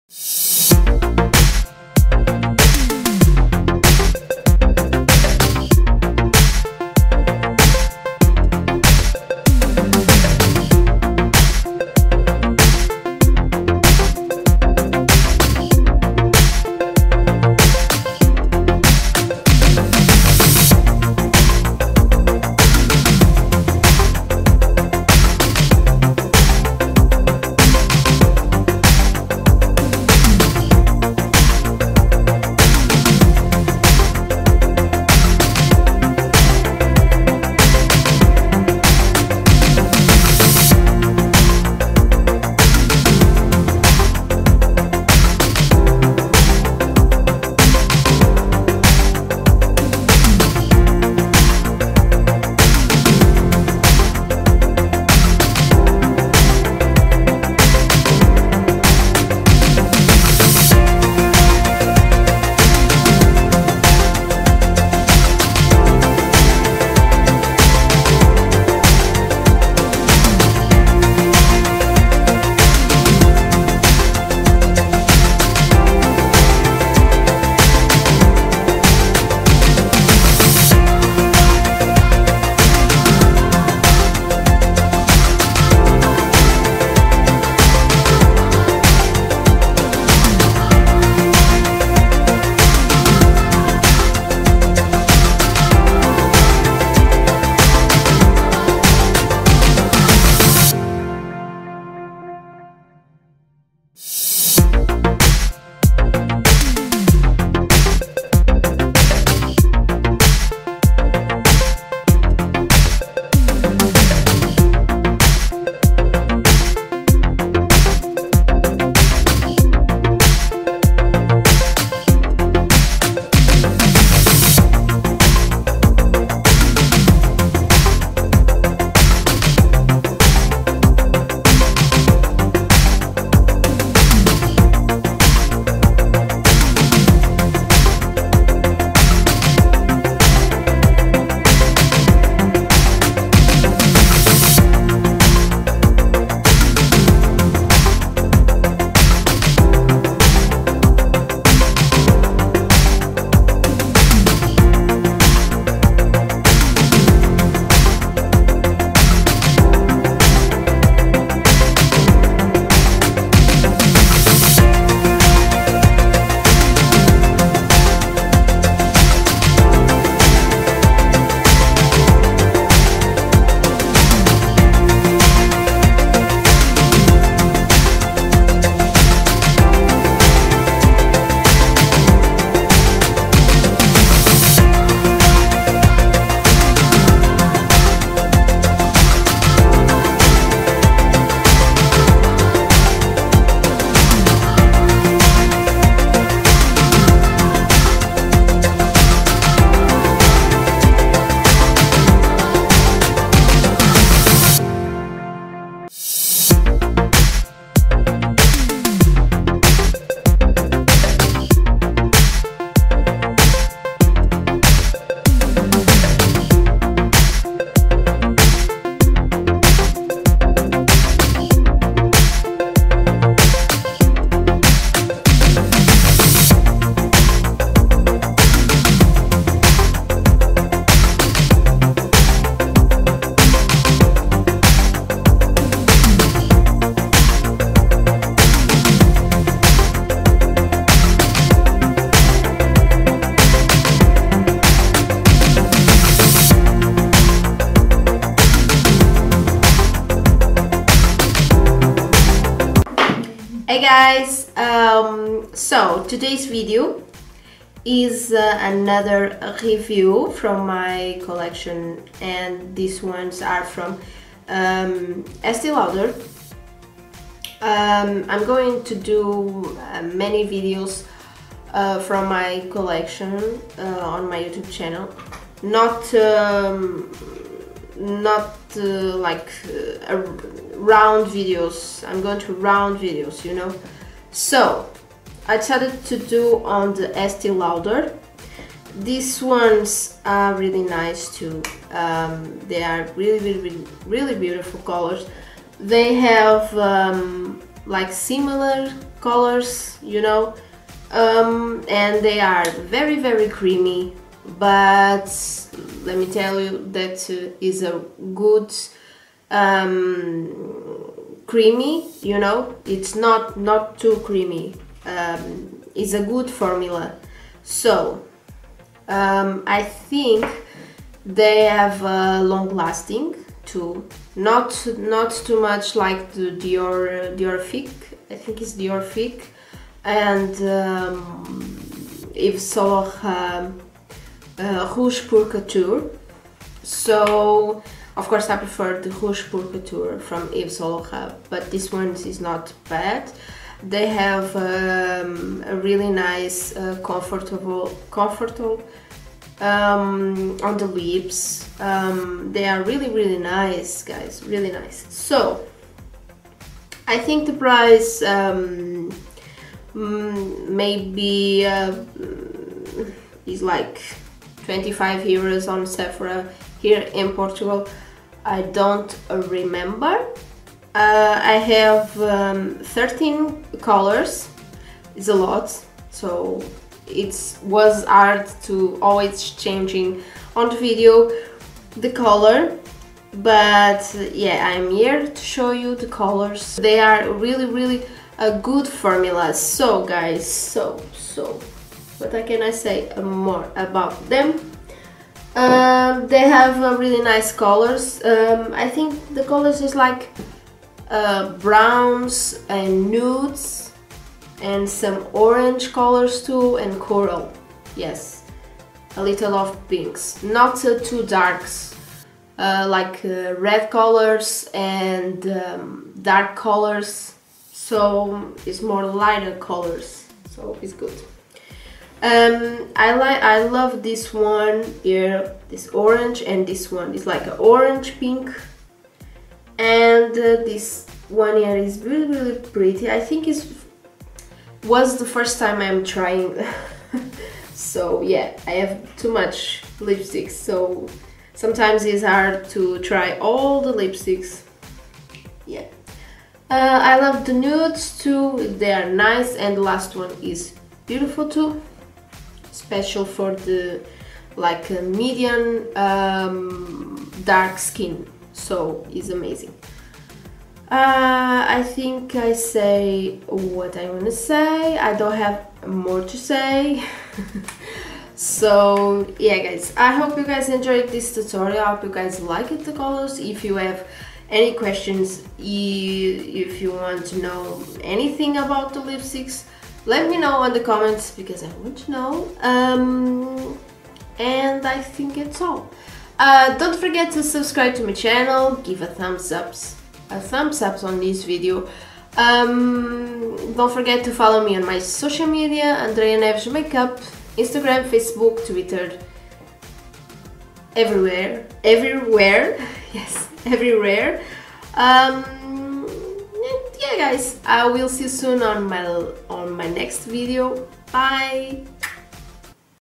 Review & Swatches: Estée Lauder Pure Color Envy
Hey loves

today i did another review from my collection lipsticks, and today is from my Estée Lauder they are really nice on the lips, and have a little shine on the colors but about that they so good quality and i totally recomand this loves.

Facebook: Andreia Neves Make Up